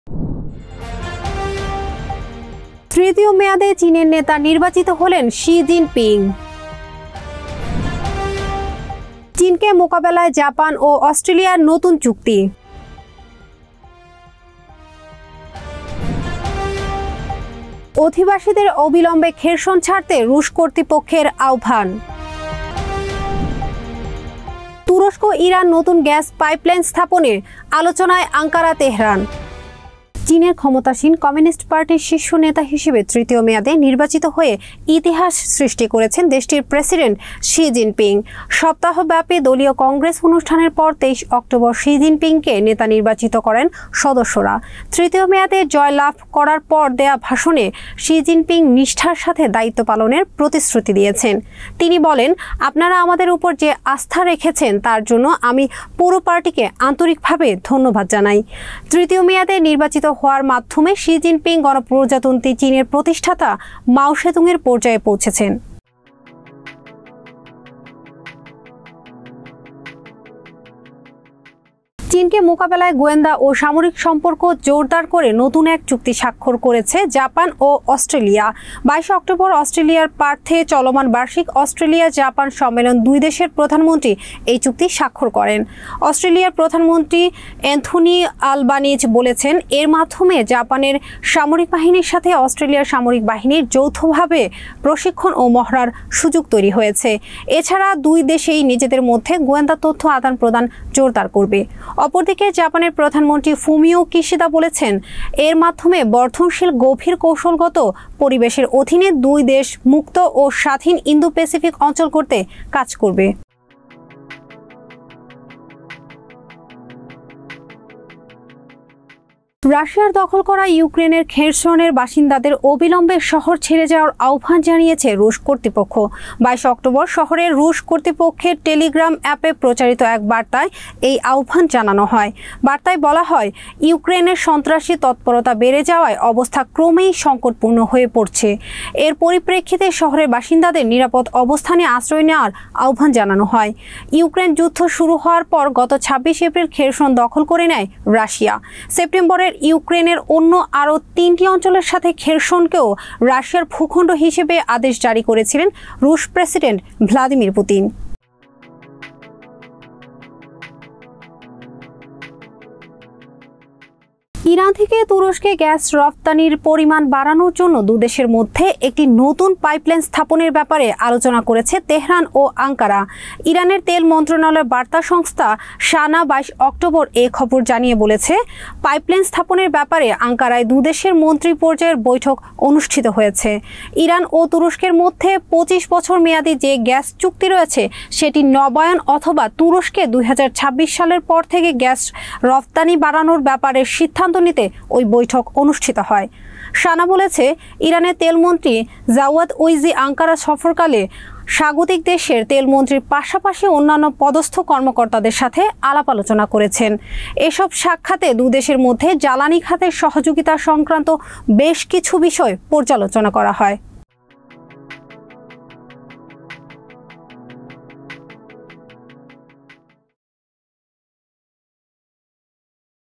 তুরস্ক-ইরানের নতুন গ্যাস পাইপলাইন । তৃতীয় মেয়াদে শি জিনপিং shi jinping
তৃতীয় মেয়াদে চীনের নেতা নির্বাচিত হলেন শি জিনপিং,
চীনকে মোকাবেলায় জাপান ও অস্ট্রেলিয়ার নতুন চুক্তি,
অধিবাসীদের অবিলম্বে খেরসন ছাড়তে রুশ কর্তৃপক্ষের আহ্বান,
তুরস্ক-ইরান নতুন গ্যাস পাইপলাইন স্থাপনে আলোচনায় আঙ্কারা-তেহরান,

1 R. K. Mission Road, Dhaka-1203.